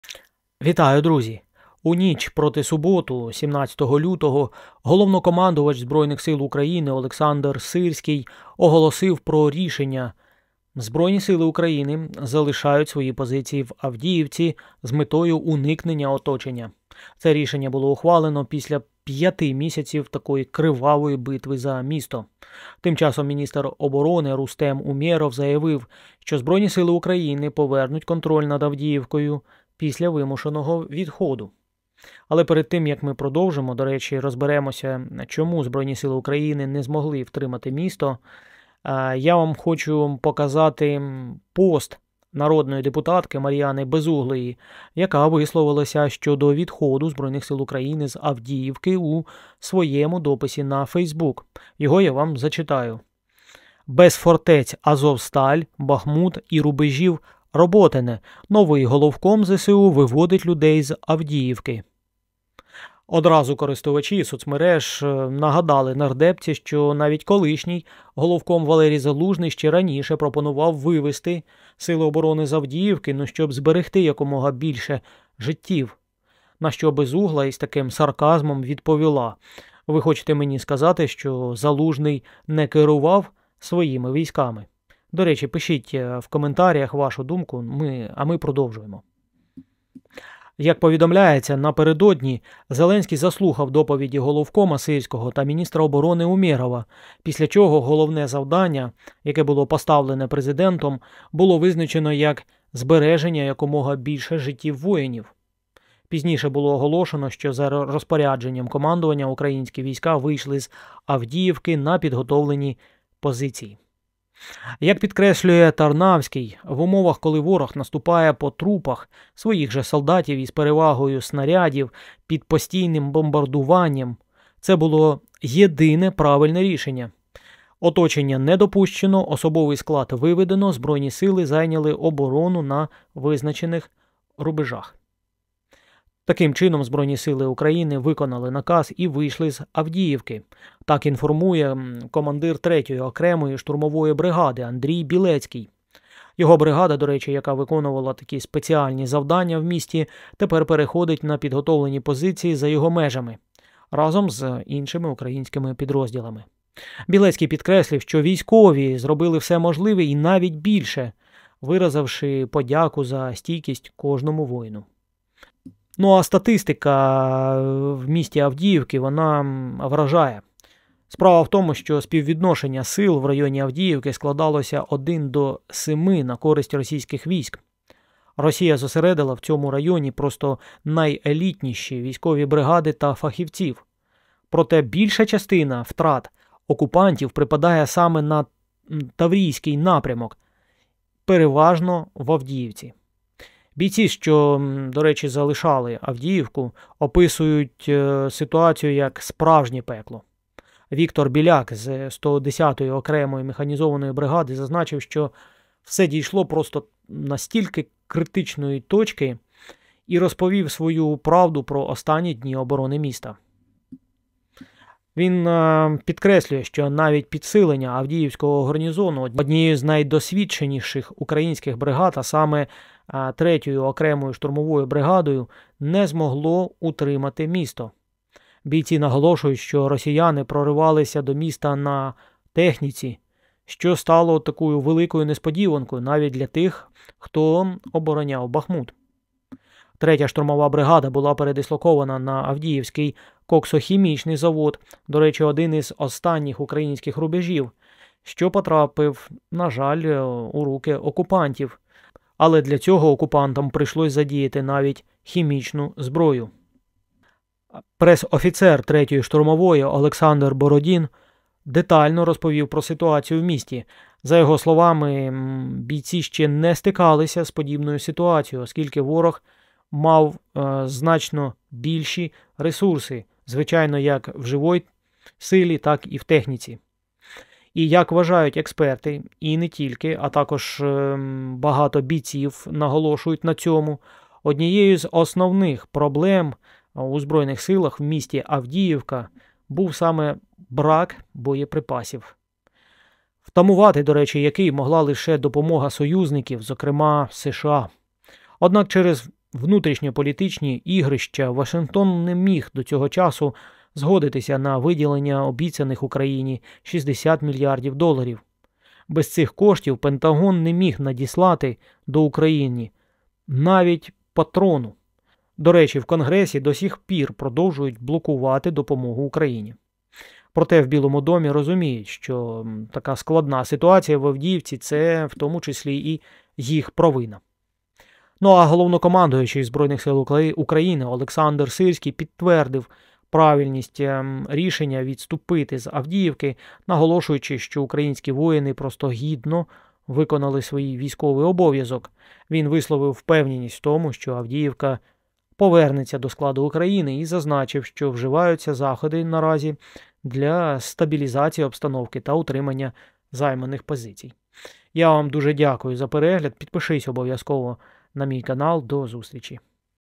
Чому не втримали Авдіївку? Як ЗСУ залишали свої позиції, Сирський наказав вийти 17.02.24
У ніч проти суботи, 17 лютого, головнокомандувач Збройних сил України, Олександр Сирський, оголосив про рішення ЗСУ залишити свої позиції в Авдіївці з метою уникнення оточення. Це рішення було ухвалено після п'яти місяців кривавої битви за місто. Тим часом, міністр оборони Рустем Умєров заявив, що Збройні сили України повернуть контроль над Авдіївкою після вимушеного відходу.